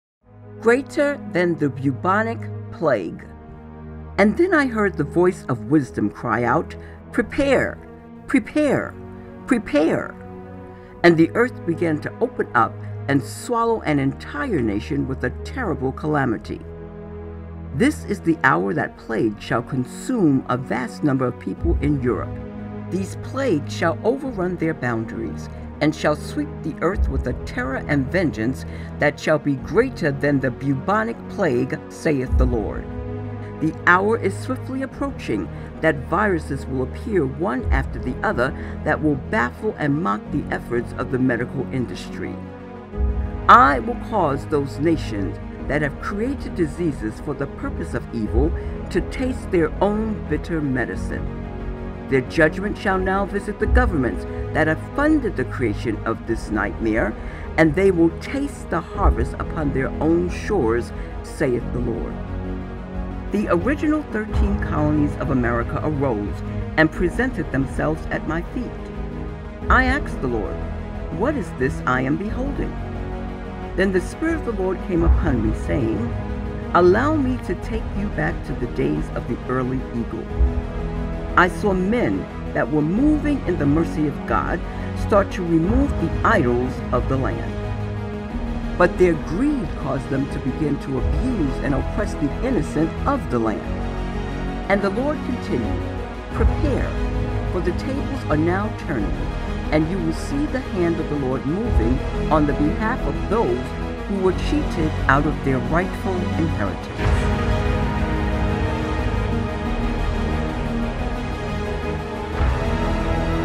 Download NOW! Written Judgments 4: Greater Than The Bubonic Plague
Master Prophet Archbishop E. Bernard Jordan prophesies about a new plague arising in this nation.

Get your copy of "Written Judgments" on zoeministries.com.